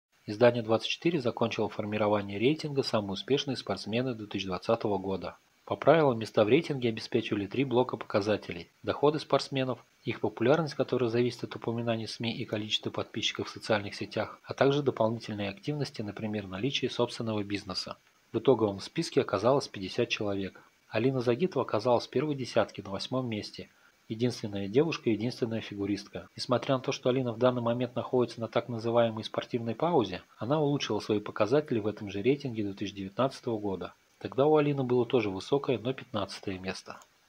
Алина Загитова вошла в рейтинг Самые успешные спортсмены 2020 года ЕДИНСТВЕННАЯ ДЕВУШКА
Алина Загитова оказалась в первой десятке в рейтинге Самые успешные спортсмены 2020